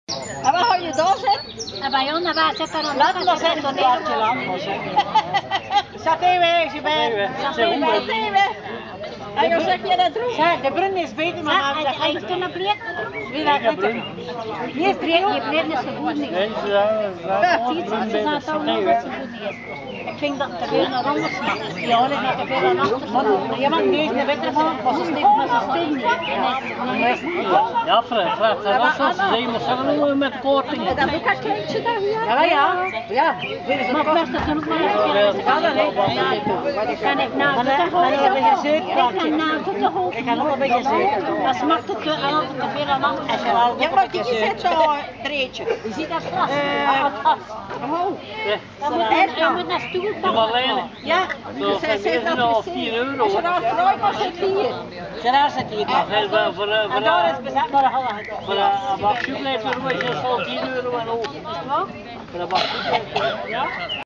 BROUWERIJ SMISJE MATER
NA DE RONDLEIDING IN DE BROUWERIJ IS HET NU GENIETEN VAN HET LEKKERE SMISKE DOOR OKRA WESTVLETEREN